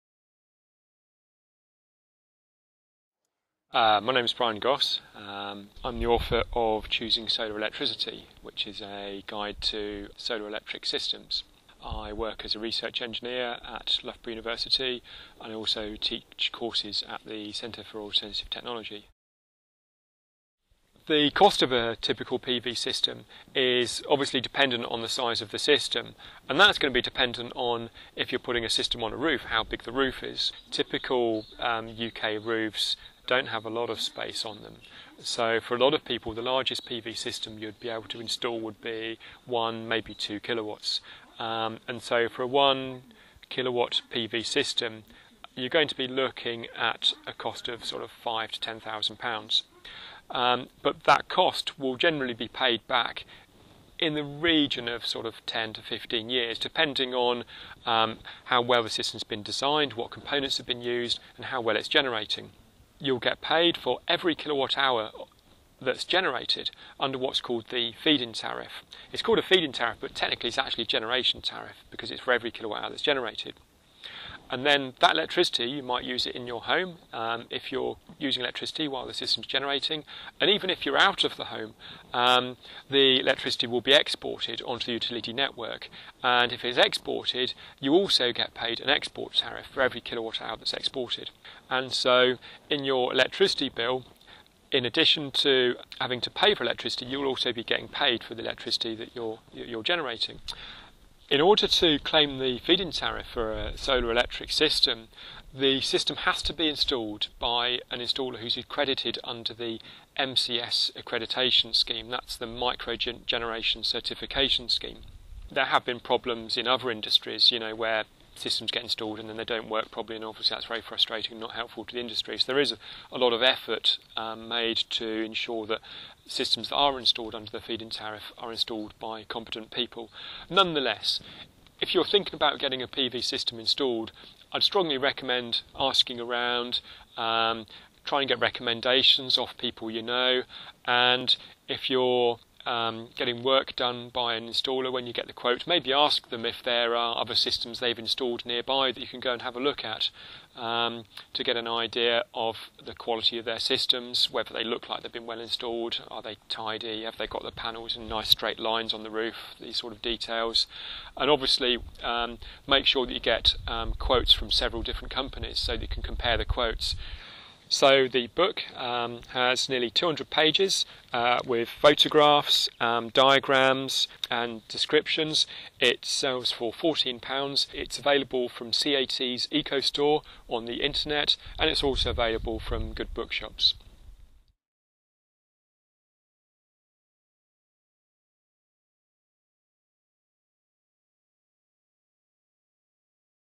Choosing Solar Electricity- Brian Goss explains installation and 'feed in tariffs'.
why should we choose solar electric? how do feed-in-tariffs FiTs work? Brian Goss explains all at the Centre for Alternative Technology

Brian Goss's book; 'Choosing Solar Electricity'.